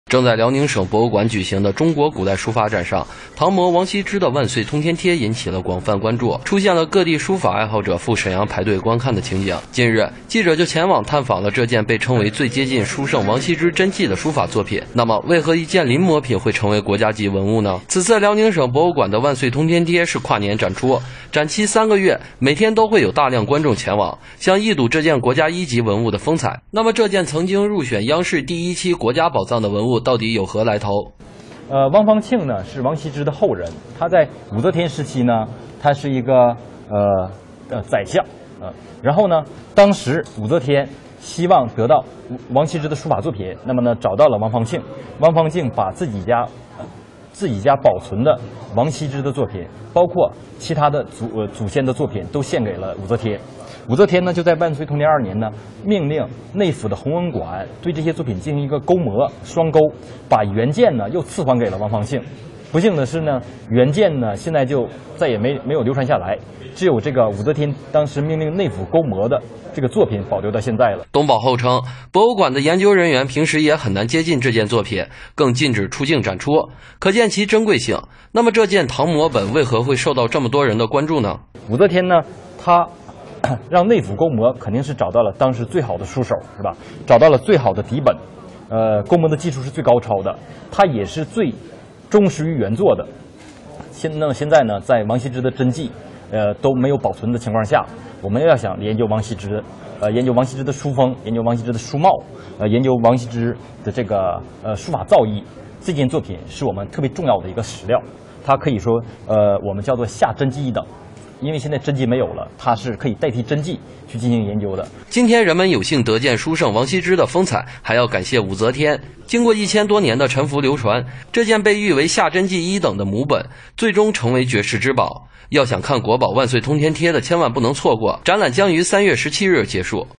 临摹王羲之真迹的《万岁通天帖》为何成了国宝？
正在辽宁省博物馆举行的“中国古代书法展”上，唐摹王羲之的《万岁通天帖》引起了广泛关注,出现了各地书法爱好者赴沈阳排队观看的情景。那么为何一件临摹品会成为国家级文物呢？记者前往探访了这件被称为最接近“书圣”王羲之真迹的书法作品。